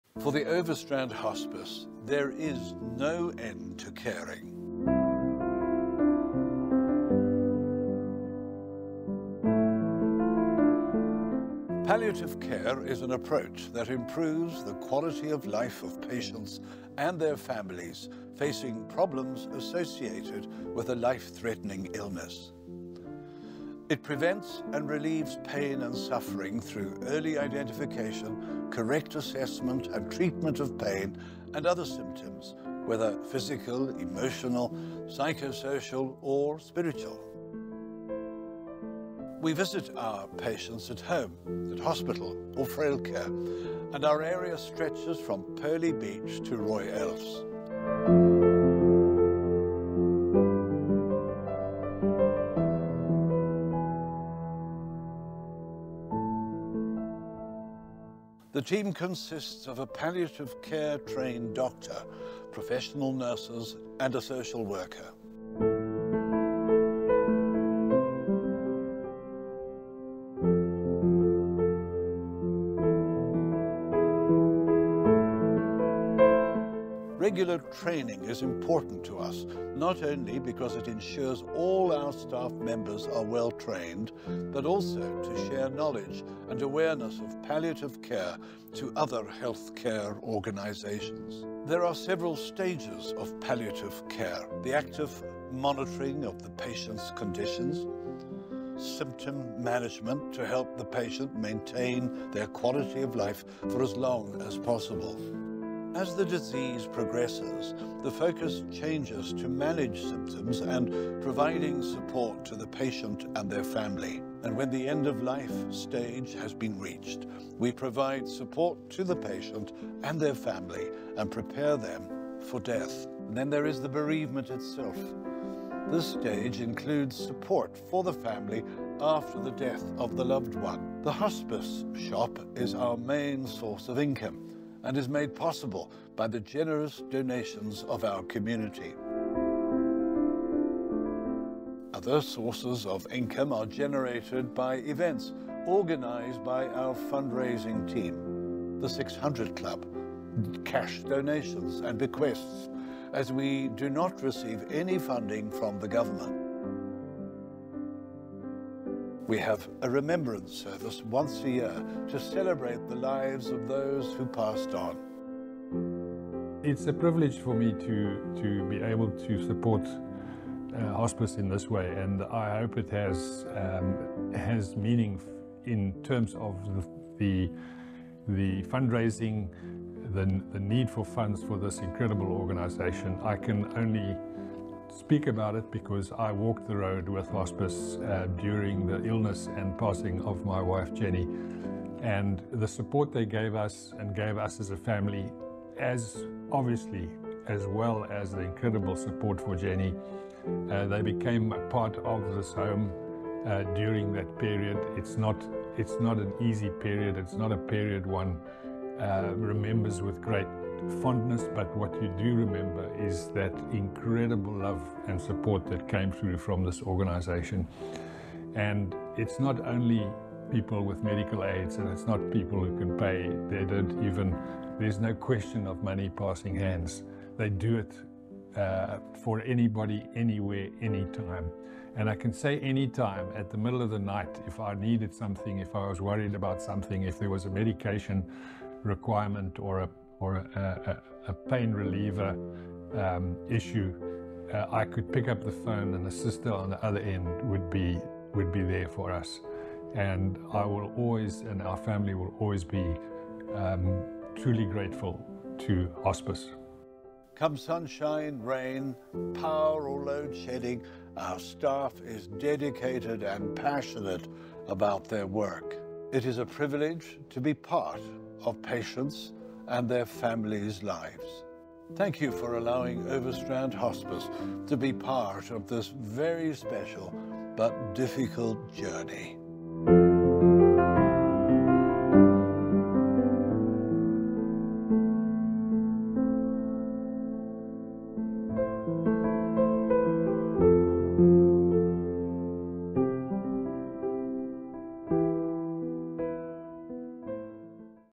Help Overstrand Hospice Keep Helping Others!
Care with compassion and dedication.

Hospice care is a specialized form of healthcare that focuses on providing comprehensive support to individuals who are in the final stages of a terminal illness. It is an essential component of the healthcare system, as it addresses not only the physical needs of patients but also their emotional and spiritual well-being.
Supporting hospice services offers several advantages, which contribute to improving the quality of life for terminally ill individuals and their families.

Firstly, hospice care provides comprehensive support by addressing the physical, emotional, and spiritual needs of patients. This holistic approach recognizes that end-of-life care should consider all aspects of a person's well-being.
By providing pain management, symptom control, counselling, and spiritual guidance, hospice services ensure that patients receive the necessary support to live their remaining days with dignity and comfort.

Secondly, effective pain and symptom management are key benefits offered by hospice care. Specialized medical interventions in hospice settings aim to alleviate pain and discomfort experienced by patients. With adequate pain management techniques in place, individuals can maintain their sense of autonomy and experience a peaceful end-of-life journey.

Furthermore, psychosocial support plays a crucial role in hospice services. Hospices offer counselling services and bereavement support not just for patients but also for their families. These interventions help individuals cope with grief, reduce anxiety levels during difficult times, and enhance overall emotional well-being.

In conclusion,
Supporting hospice services come with numerous advantages.
By providing comprehensive care, hospices address multiple dimensions
of patient well-being, leading to improved quality of life for both patients
and their loved ones. The significance of promoting access to quality
end-of-life care through continued support for hospice programs cannot be overstated.
It is imperative that society recognizes the importance of hospice care and continues to provide resources and support to ensure that terminally ill individuals receive the comfort, dignity, and respect they deserve in their final days.

Well worth any support you are able to assist with.